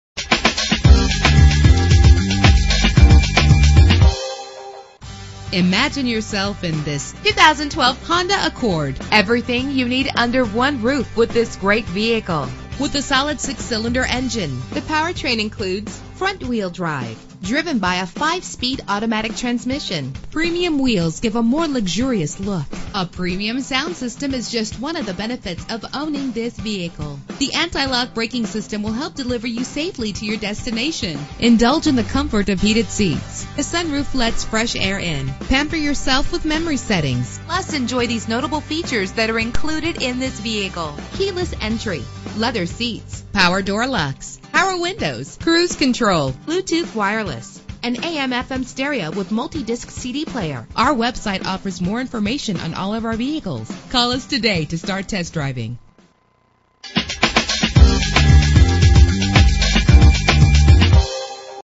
2012 Honda Accord - St. Petersburg FL
Year: 2012
Make: Honda
Model: Accord
Trim: EX-L
Engine: 3.5 liter 6 cylinder 24 valve
Transmission: 5-Speed Automatic
Color: Alabaster Silver Metallic
Mileage: 29094
Address:
5301 34th St N